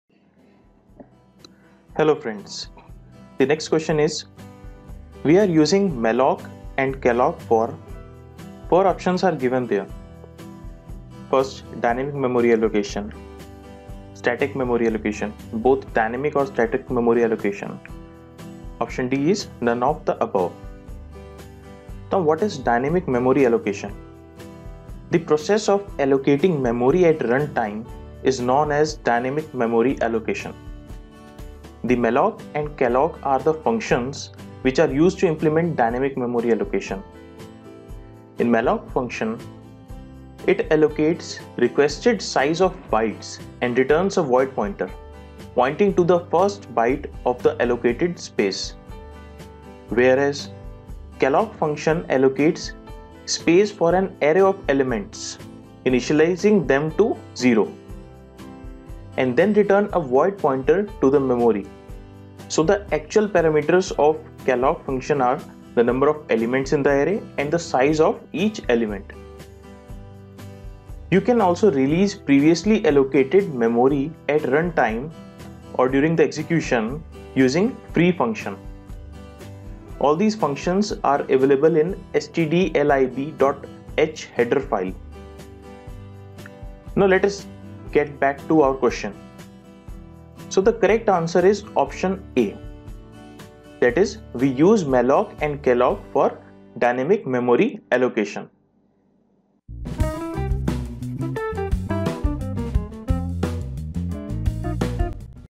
We use malloc and calloc for
(a) Dynamic memory allocation
(b) Static memory allocation

ISRO 2017 CS Solution || Watch Freely on your Laptops, Tablet, smartphones ( Windows, Android, iPhone )